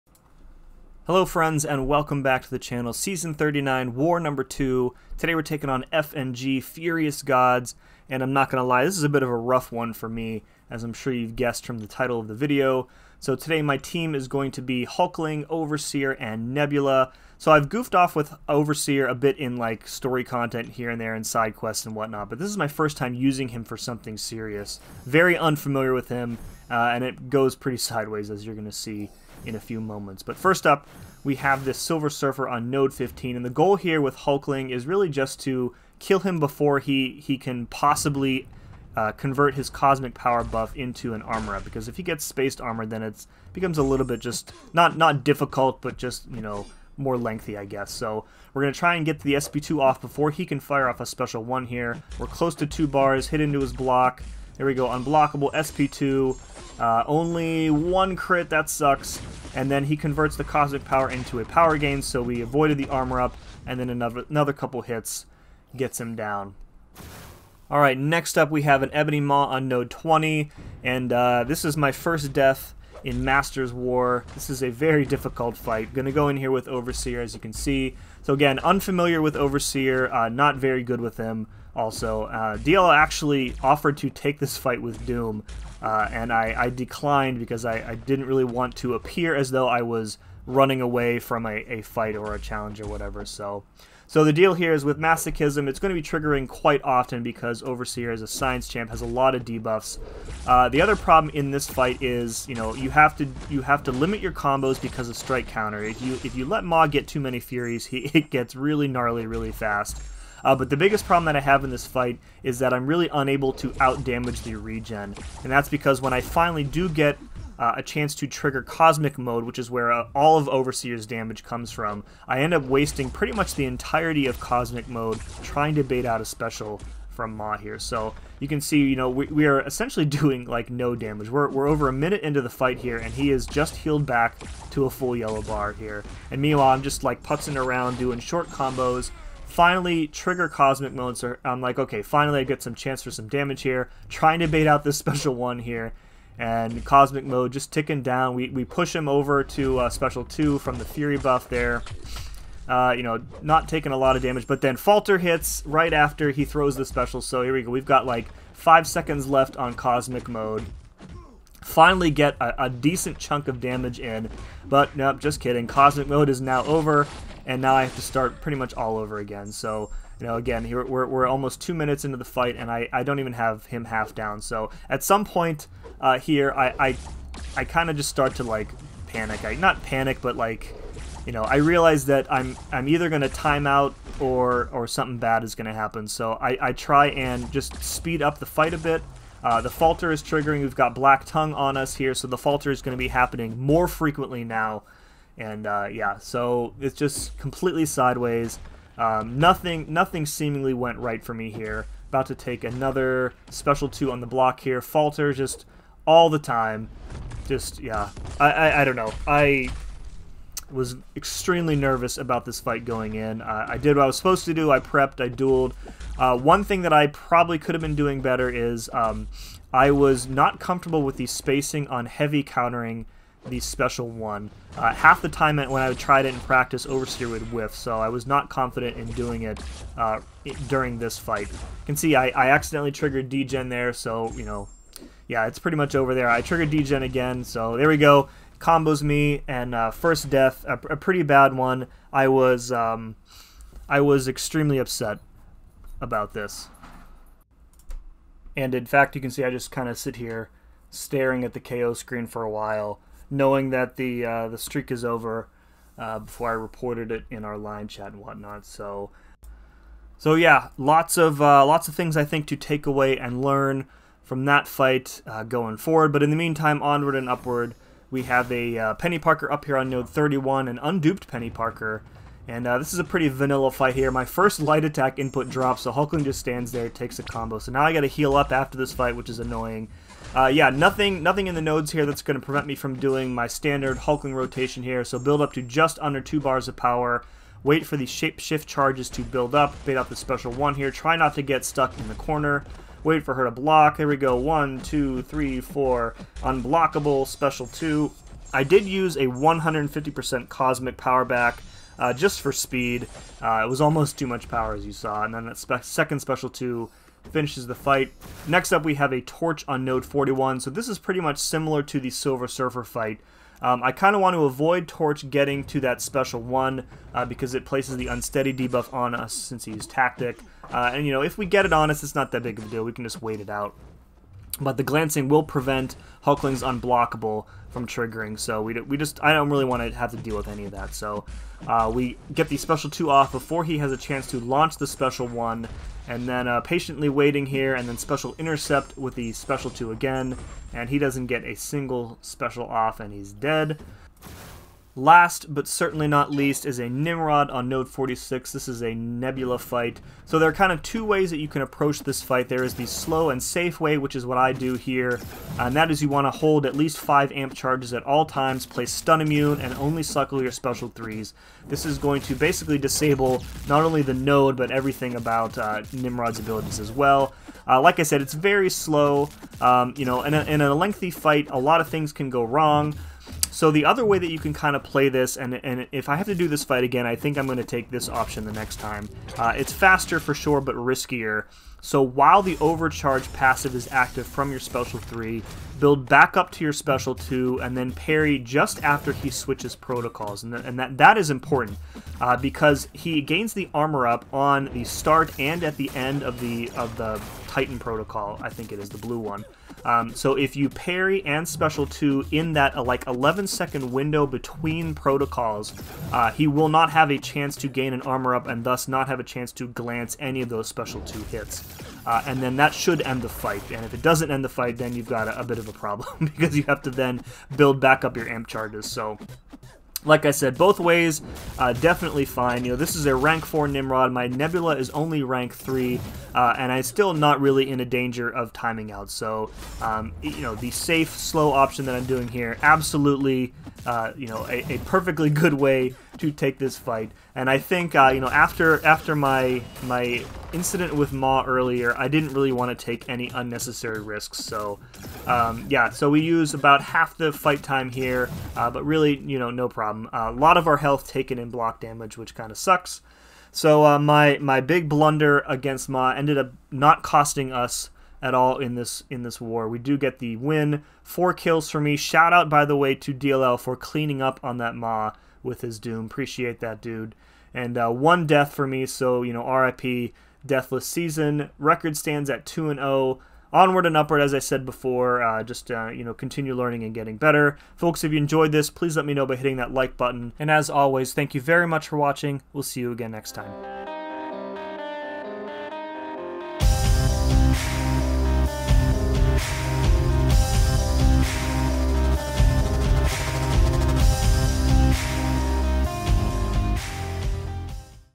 Trying to Channel BMcG but Dying Horribly | Season 39, War 02
00:00 - Intro and Team Assignment
00:21 - Node 15 // SILVER SURFER (Hulkling)
01:05 - Node 20 // EBONY MAW (Overseer)
04:26 - Node 31 // PENI PARKER (Hulkling)
05:06 - Node 41 // HUMAN TORCH (Hulkling)
05:43 - Node 46 // NIMROD (Nebula)
07:30 - War Results

Donations are never expected but always appreciated and help me keep doing what I do!

🎵 THE SWEET, SWEET TUNES YOU HEARD IN THIS VIDEO 🎵